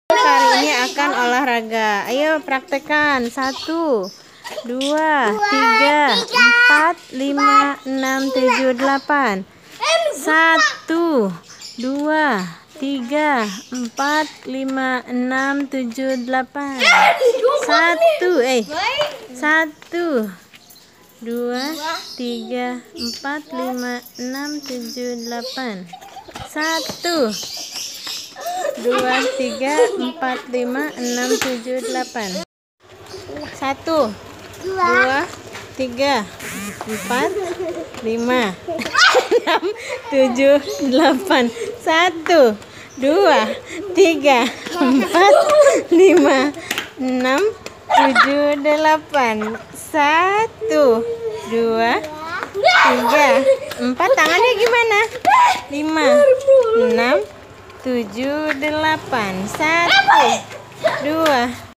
Olahraga ditengah pandemi Covid-19
Olahraga gaya kami pada saat pertama kali ada peraturan stay at home